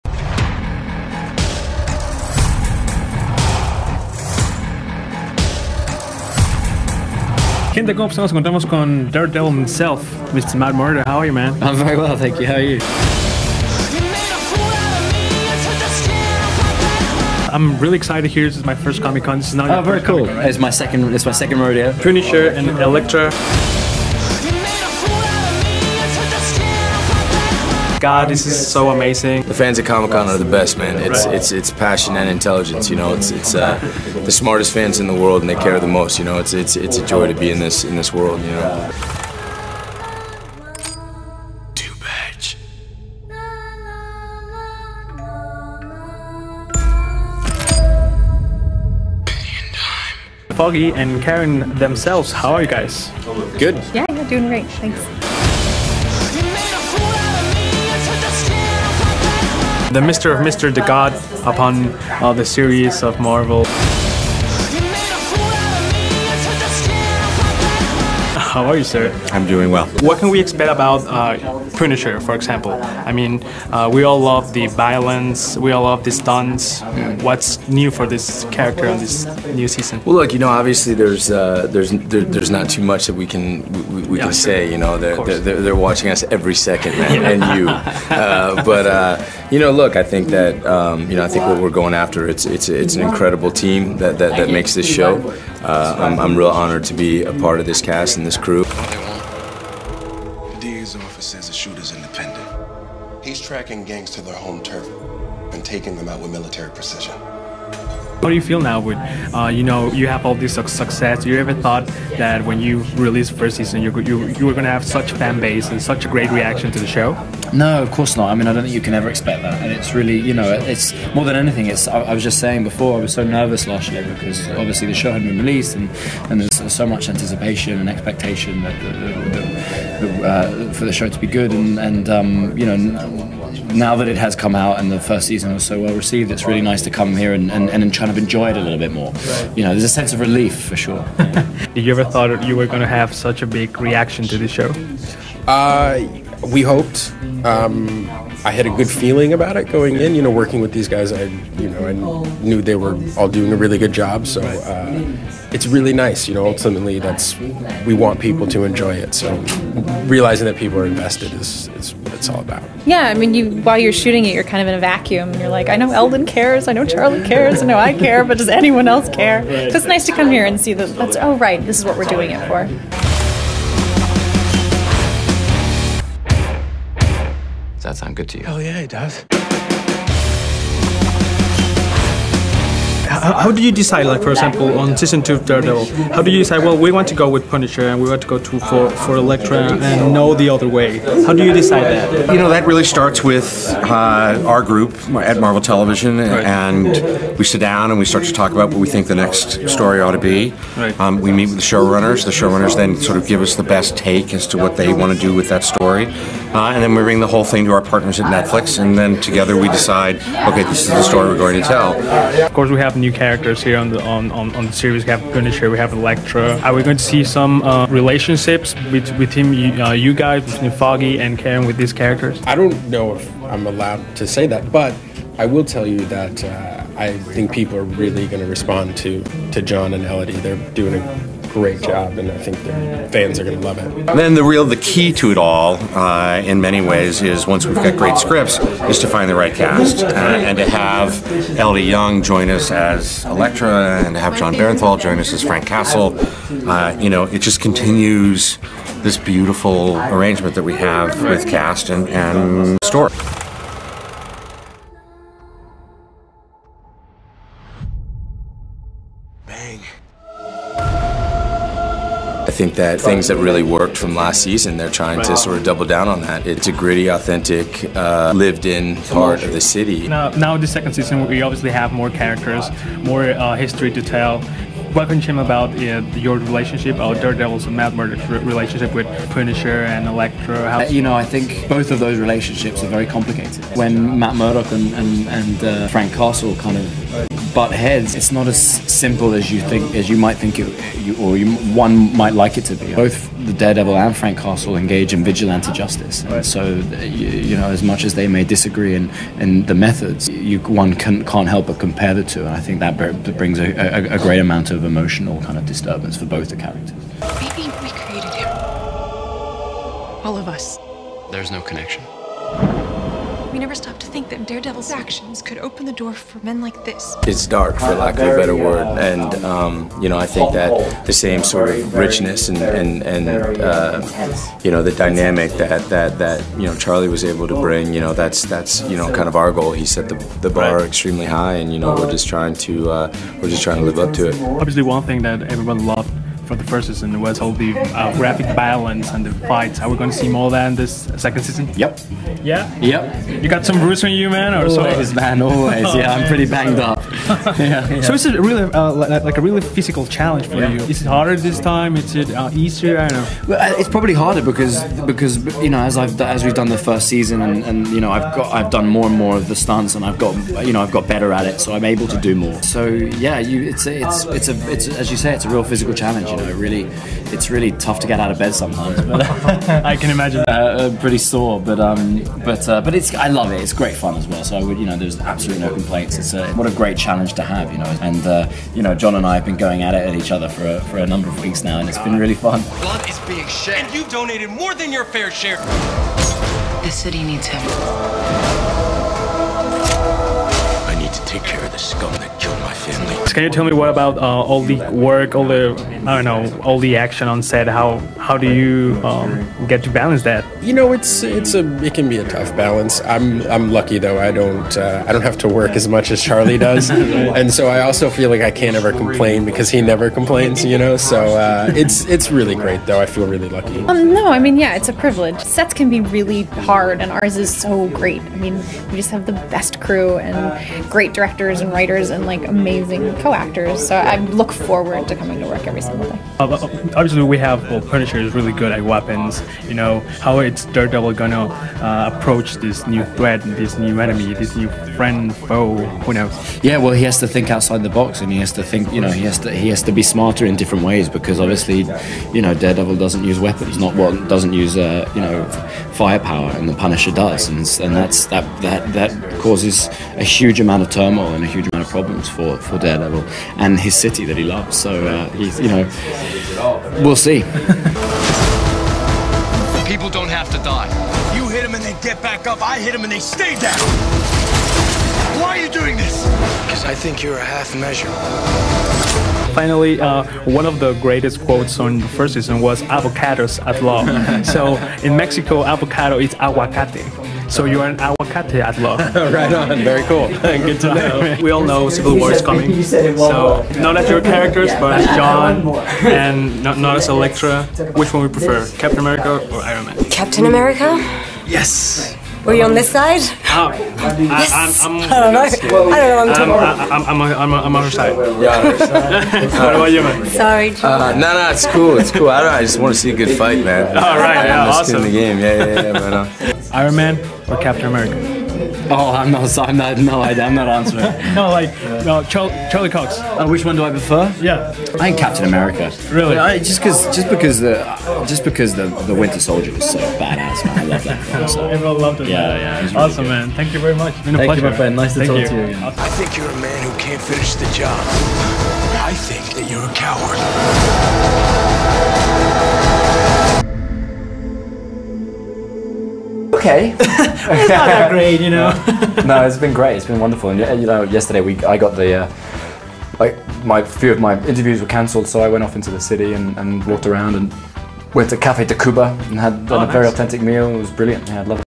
Close Up Daredevil Season 2 (entrevistas exclusivas)
Nos lanzamos a la COMIC-CON de NUEVA YORK para platicar con los PROTAGONISTAS de la 2da TEMPORADA de DAREDEVIL.